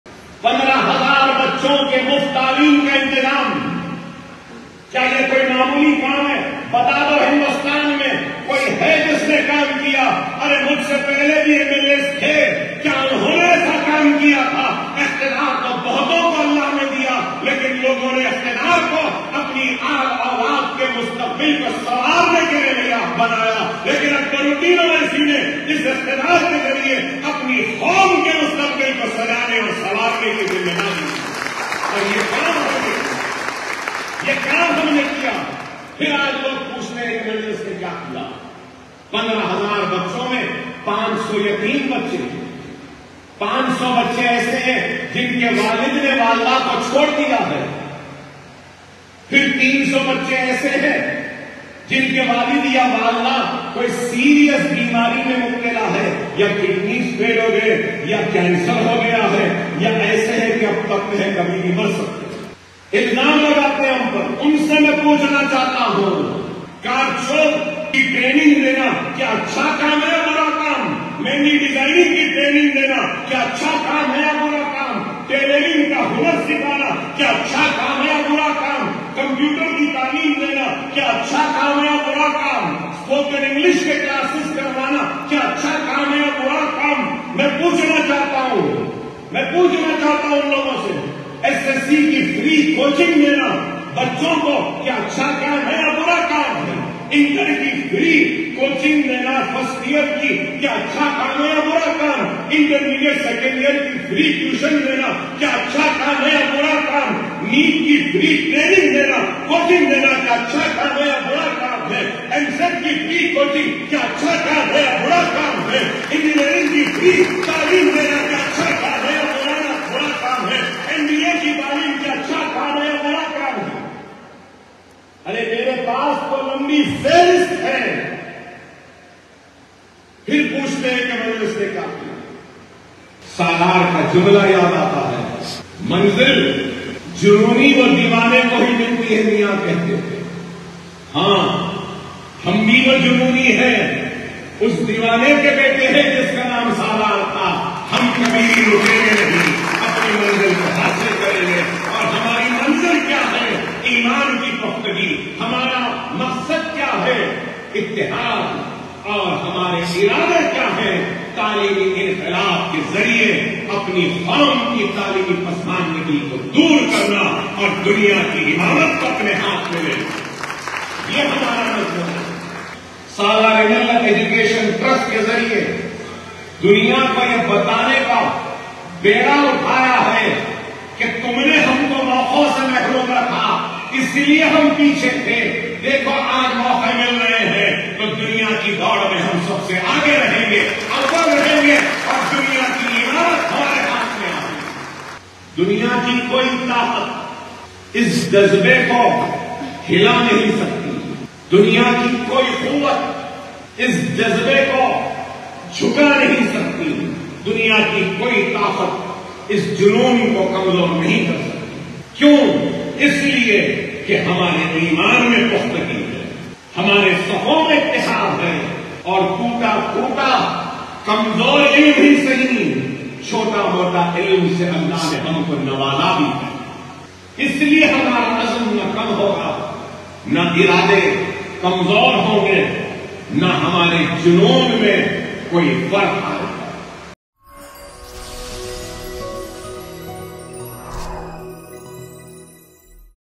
Kya Yeh Acha Kaam Nahi Hai? Akbar Owaisi Ka Sawal
AkbaruddinOwaisi distributed free vocational course certificates in tailoring (with sewing machines), Mehndi designing, beautician (with kits), embroidery, computer training, spoken English, adult education, and other women empowerment courses today at Fatima Owaisi Educational Campus KG to PG in Bandlaguda.